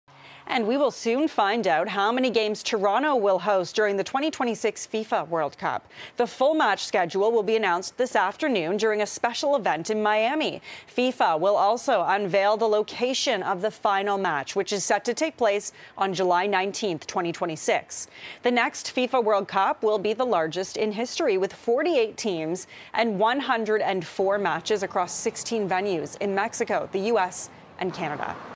2026 World Cup schedule to be announced today
The full match schedule will be announced this afternoon during a special event in Miami.